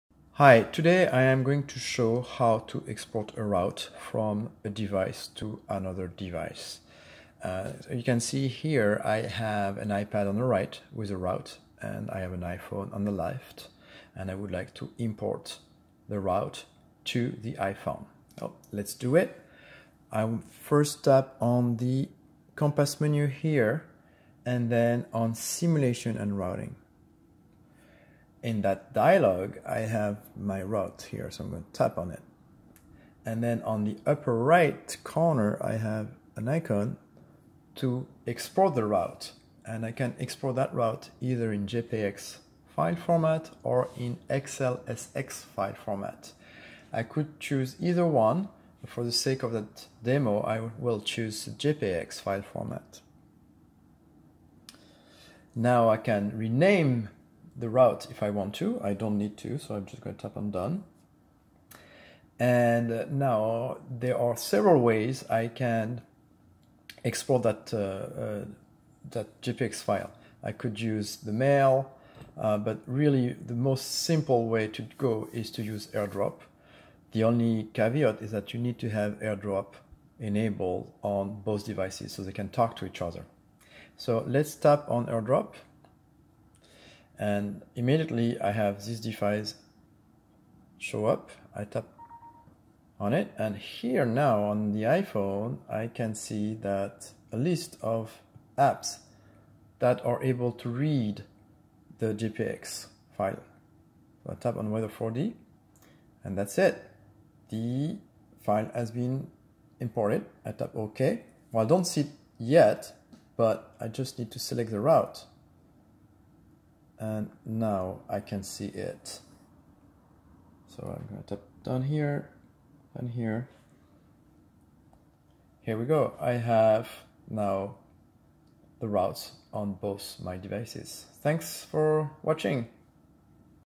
Exporting and importing a route
How to export a route from a device to import it to another device using AirDrop.